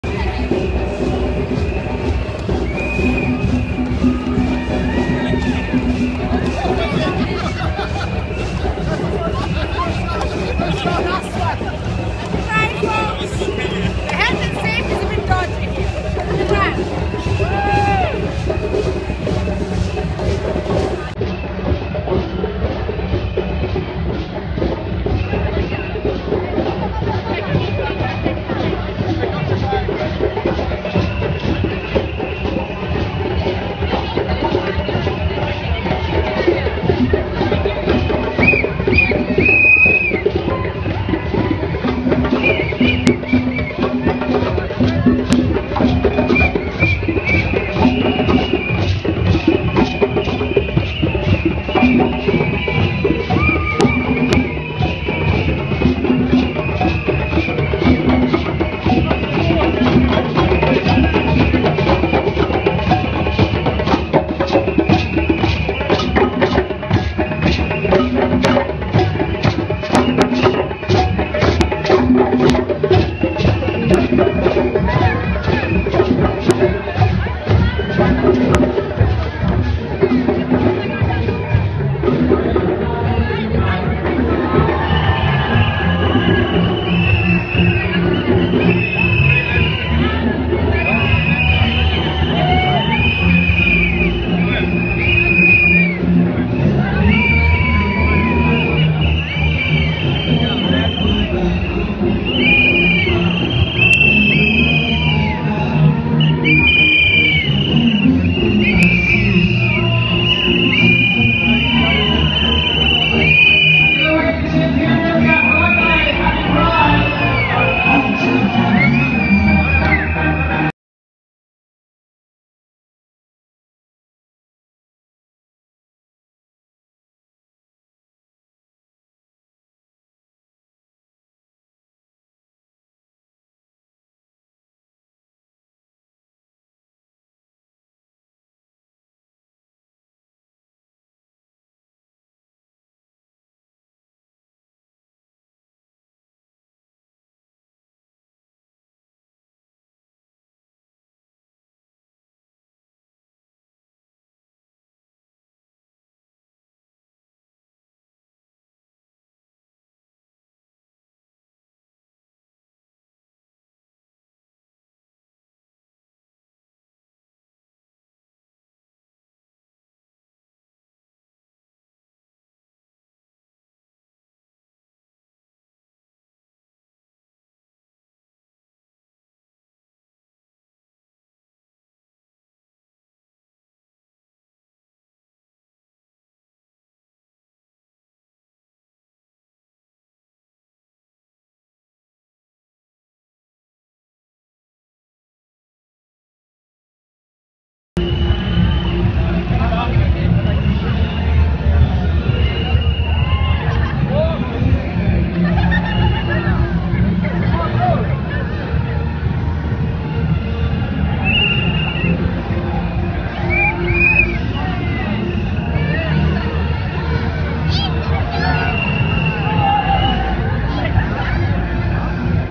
Cork Pride parade August 3, 2014.
Cork Pride Parade down the South Mall on August 3, 2014.